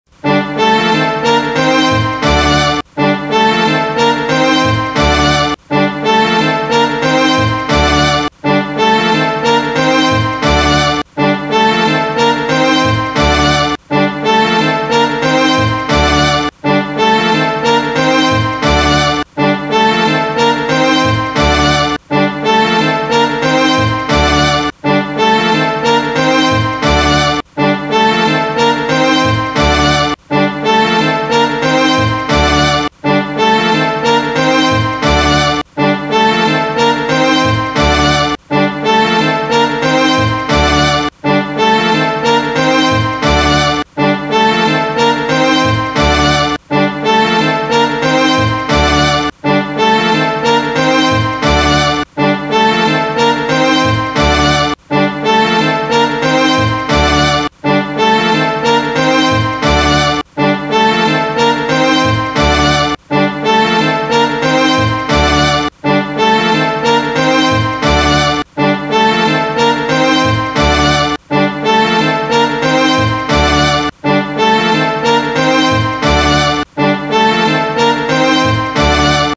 [#294] Messing Around With Logos | Episode 204 | WETA Washington, D.C. (1989)
Effects on the 1989 WETA Washington, D.C. logo.